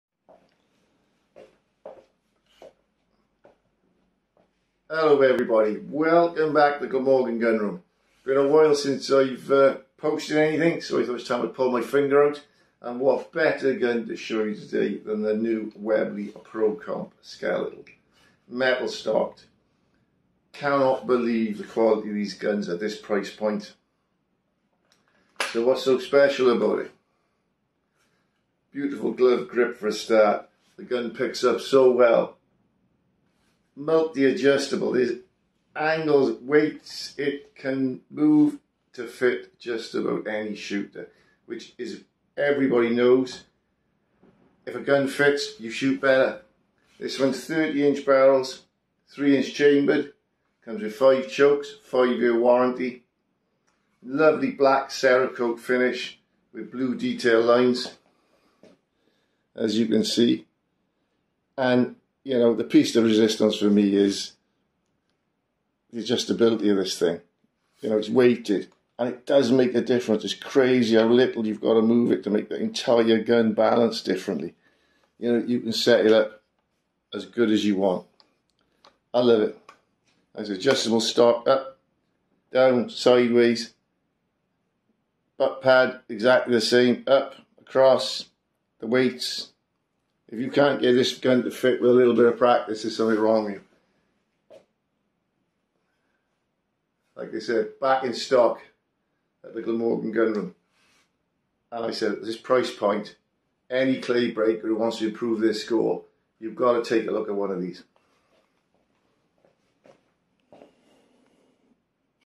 The new Webley & Scott pro comp metal stock.
The bargain clay breaker of the current competitive shooting 12g market.
30 inch Barrels,steel proofed 3 inch chamber, single trigger, glove grip, cerakote black finish with blue detail. totally adjustable stock a true engineering master piece.@glamorgangunroom191#webley&scott#webleyandscott#highlandoutdoors#clayshooters#,shotgunsports#clayshooting#claybreakers